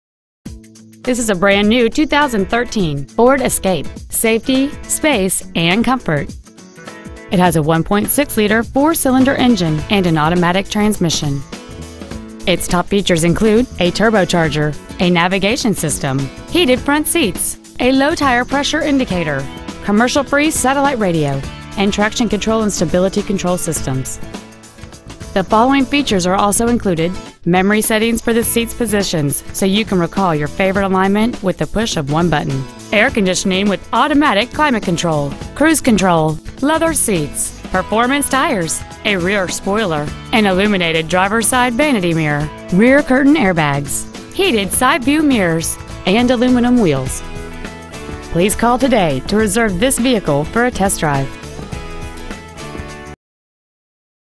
2013 Ford Escape Chattanooga TN
Year : 2013
 Make : Ford
 Model : Escape
 Engine : 1.6L I4
 Trans . : Automatic
 Exterior : Gray

 Interior : Gray
 Marshal Mize Ford